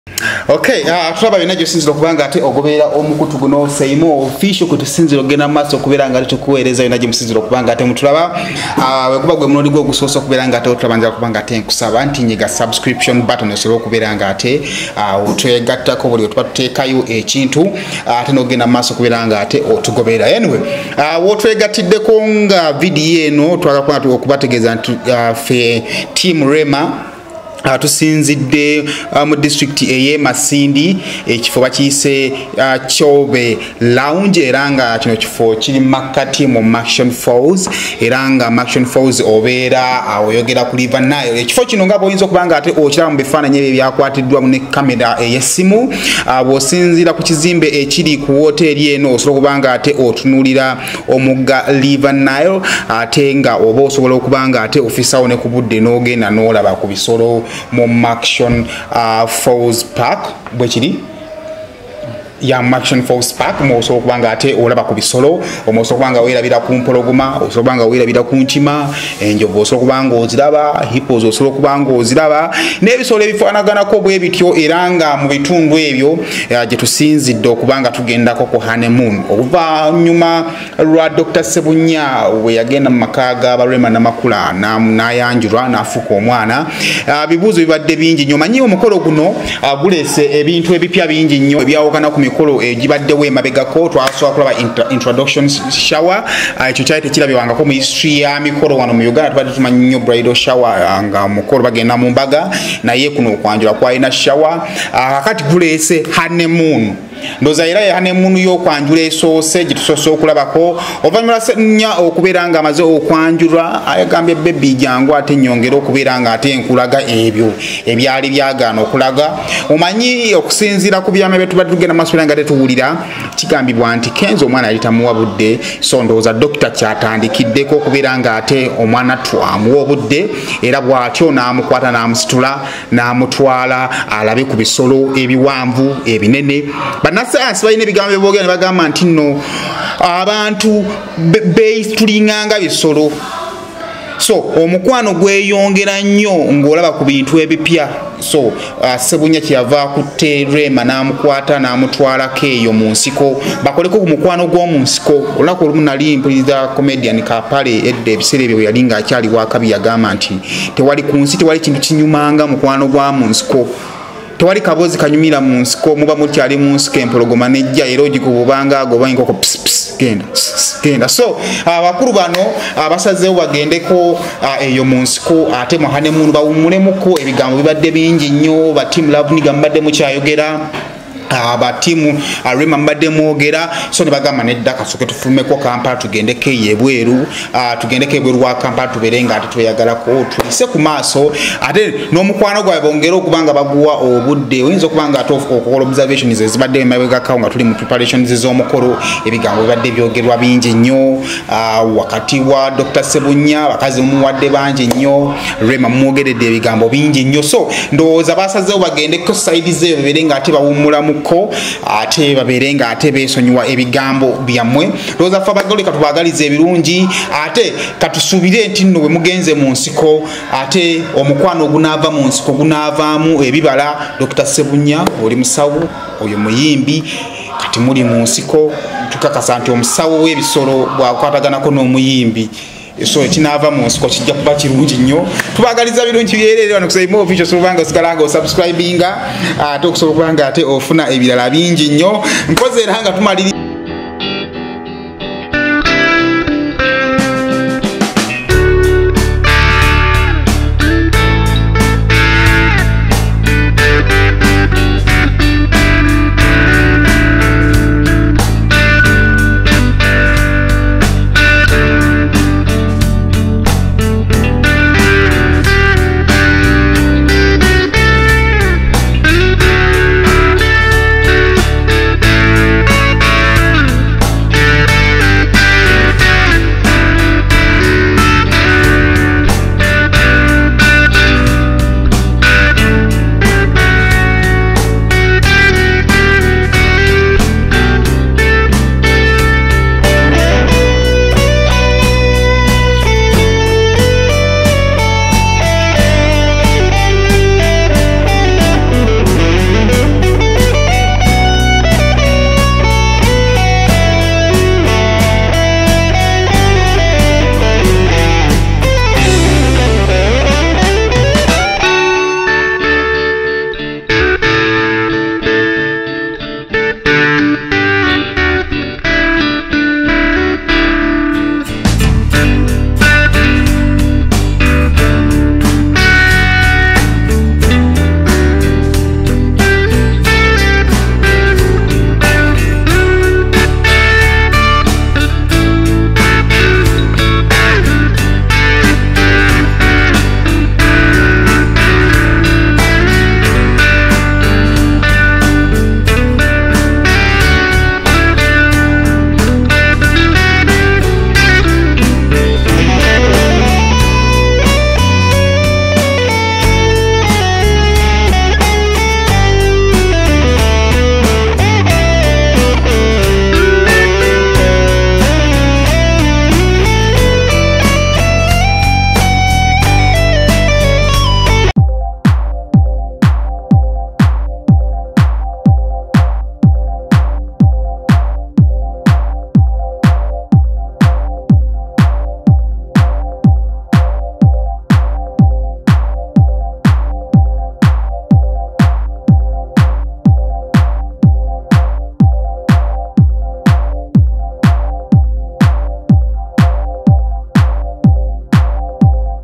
Dr. Ssebunya ayongedde obukodyo mu mukwano gwe ne Rema. Honeymoon bagitutte munsiko.
Yiino Honeymoon ya Rema Ne Dr. Hamza Omukwano bagututte munsiko.